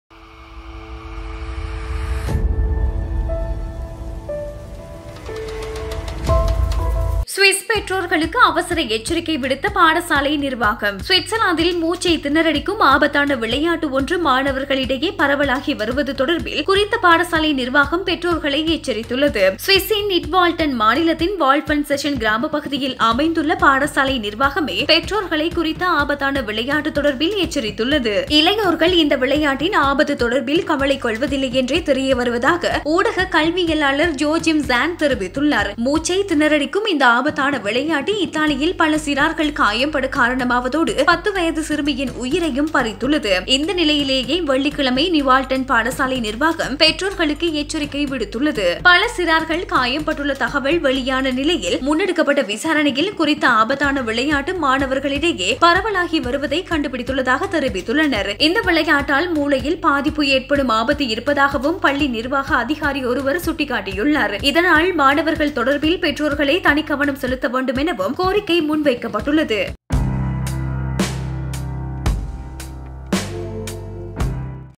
சுவிஸ் பெற்றோர்களுக்கு அவசர எச்சரிக்கை விடுத்த பாடசாலை நிர்வாகம்!
சுவிட்சர்லாந்தில் மூச்சைத்திணறடிக்கும் ஆபத்தான விளையாட்டு ஒன்று மாணவர்களிடையே பரவலாகி வருவது தொடர்பில் குறித்த பாடசாலை நிர்வாகம் பெற்றோர்களை எச்சரித்துள்ளது.

சுவிஸின் நிட்வால்டன் மாநிலத்தின் Wolfenschiessen கிராமப்பகுதியில் அமைந்துள்ள பாடசாலை நிர்வாகமே பெற்றோர்களை குறித்த ஆபத்தான விளையாட்டு தொடர்பில் எச்சரித்துள்ளது.